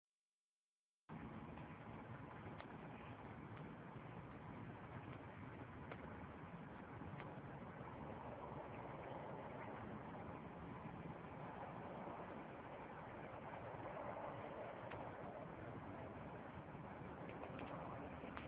Confused Dragonflies
The speck in the middle is two dragonflies trying to lay eggs on a black car.